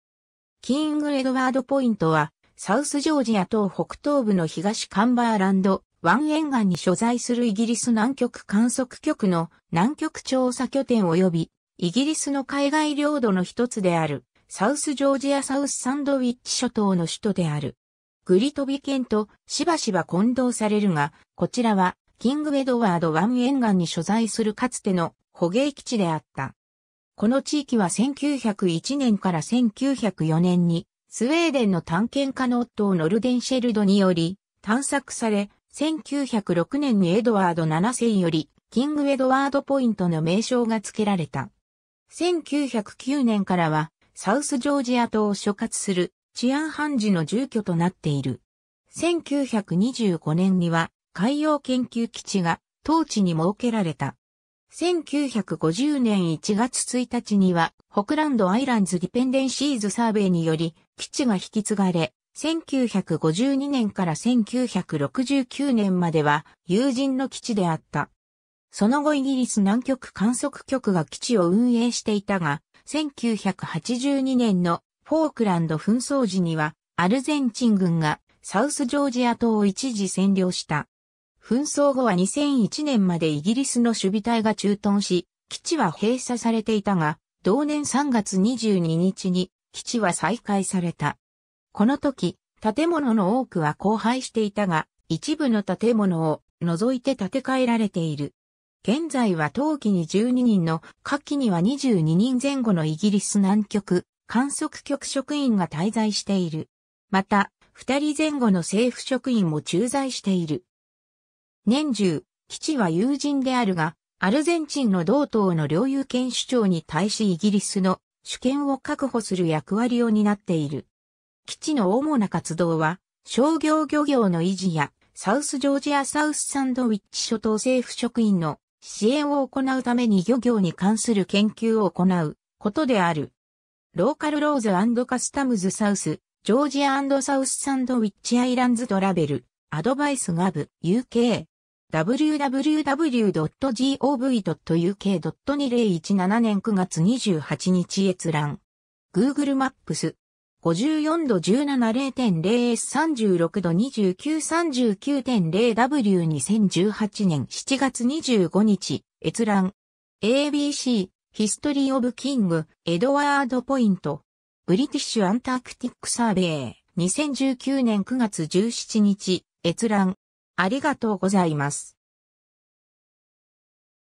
キング・エドワード・ポイント
サウスジョージア・サウスサンドウィッチ諸島
キング・エドワード・ポイント (英語: King Edward Point)はサウスジョージア島北東部の東カンバーランド湾沿岸に所在するイギリス南極観測局の南極調査拠点およびイギリスの海外領土の一つであるサウスジョージア・サウスサンドウィッチ諸島の首都である。
 グリトビケンとしばしば混同されるが、こちらはキング・エドワード湾沿岸に所在するかつての捕鯨基地であった。
 この地域は1901年から1904年にスウェーデンの探検家のオットー・ノルデンシェルドにより探索され、1906年にエドワード7世よりキング・エドワード・ポイントの名称が付けられた。
1909年からはサウスジョージア島を所轄する治安判事の住居となっている。
1925年には海洋研究基地が当地に設けられた。
 1950年1月1日にはFalkland Islands Dependencies Surveyにより基地が引き継がれ、1952年から1969年までは有人の基地であった。
その後イギリス南極観測局が基地を運営していたが、1982年のフォークランド紛争時にはアルゼンチン軍がサウスジョージア島を一時占領した。
紛争後は2001年までイギリスの守備隊が駐屯し基地は閉鎖されていたが、同年3月22日に基地は再開された。
この時、建物の多くは荒廃していたが、一部の建物を除いて建て替えられている。
 現在は冬季に12人の、夏季には22人前後のイギリス南極観測局職員が滞在している。
また、2人前後の政府職員も駐在している。
年中、基地は有人であるがアルゼンチンの同島の領有権主張に対しイギリスの主権を確保する役割を担っている。
基地の主な活動は商業漁業の維持やサウスジョージア・サウスサンドウィッチ諸島政府職員の支援を行うために漁業に関する研究を行うことである。
^ “Local laws and customs - South Georgia and South Sandwich Islands (British Overseas Territory) travel advice - GOV.UK” (英語). 2017年9月28日閲覧。
 ^ “Google Maps” (英語). 54°17'00.0"S 36°29'39.0"W. 2018年7月25日閲覧。
 ^ a b c “History of King Edward Point (Station M)”. British Antarctic Survey. 2019年9月17日閲覧。